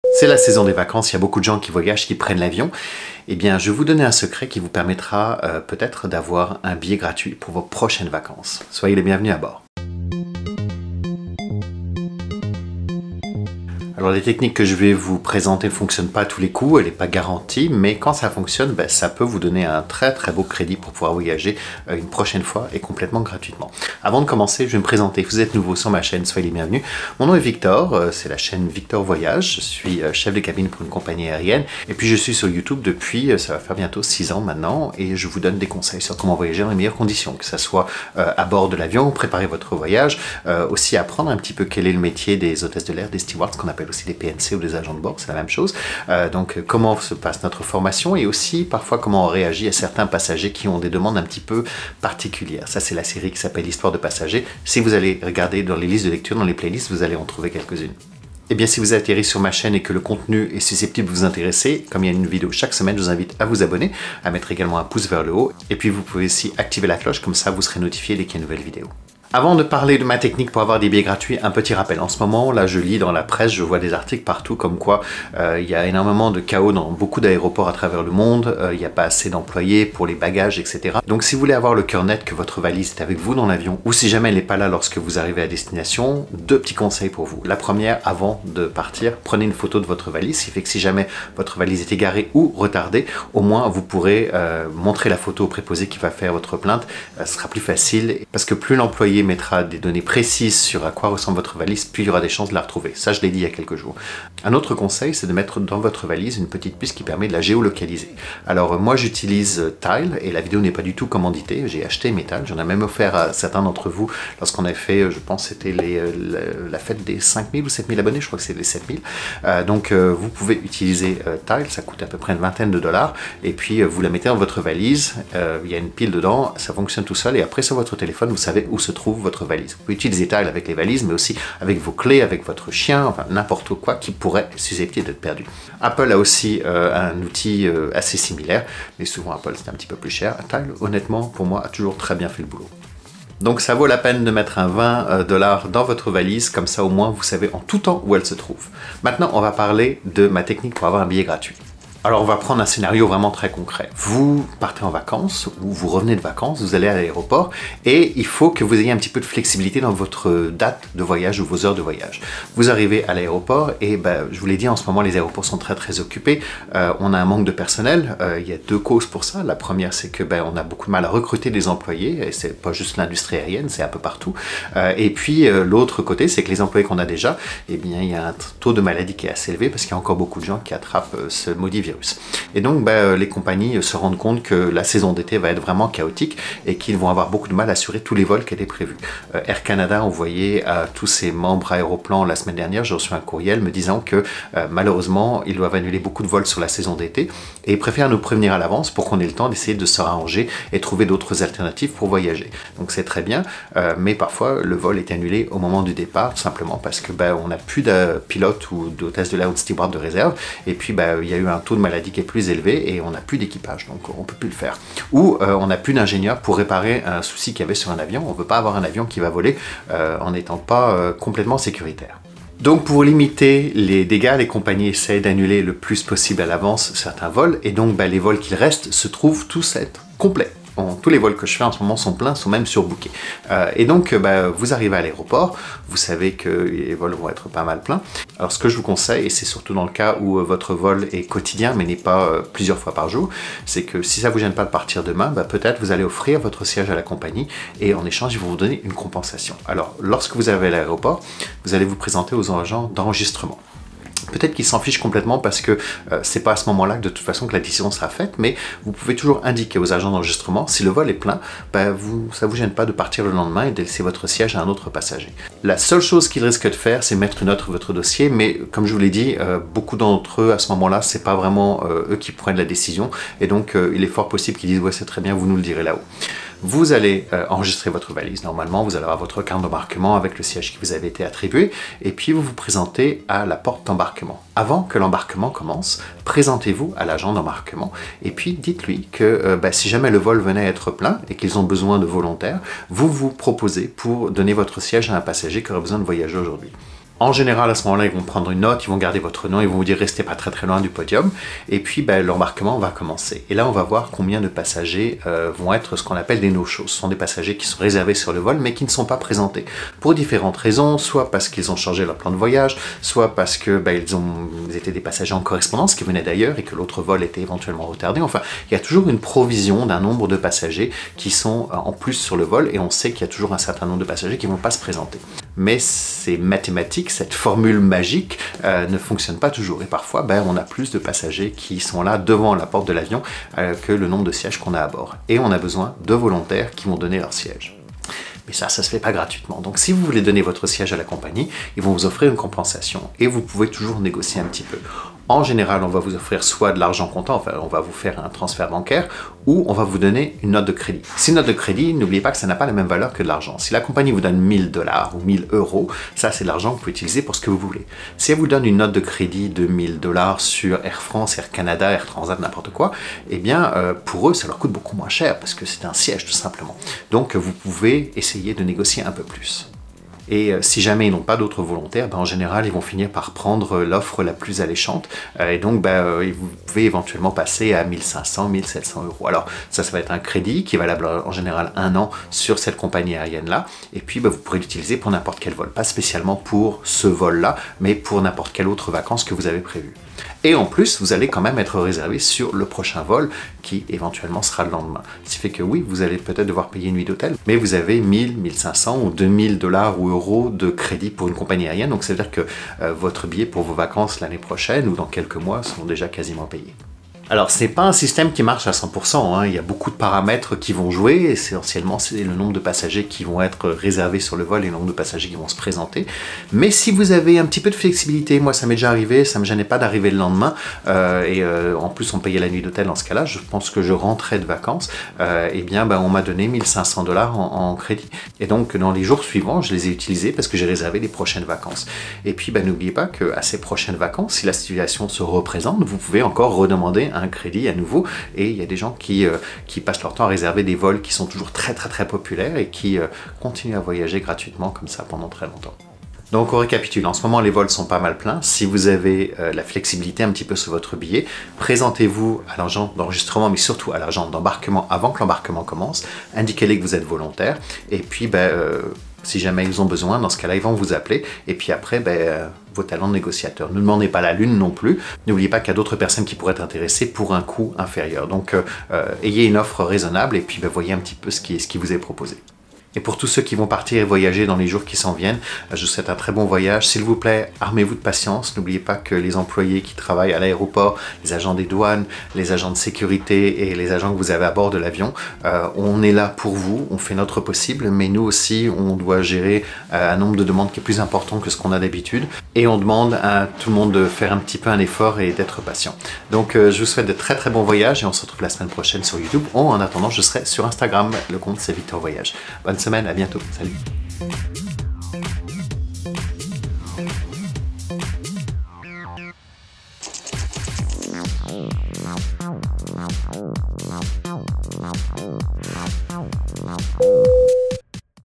TECHNIQUE POUR VOLER GRATUITEMENT | Victor Voyage
Il existe une technique pour obtenir des credits voyages avec les compagnies aériennes. Ce système n'est jamais garantit, mais fonctionne tres bien en ces temps ou les vols sont complets

Time Code:
0:00 Intro
1:09 Tile
2:32 Scénario et contexte
3:58 Flexibilité
4:41 Se faire connaitre
5:40 Compensation

Bonne chance!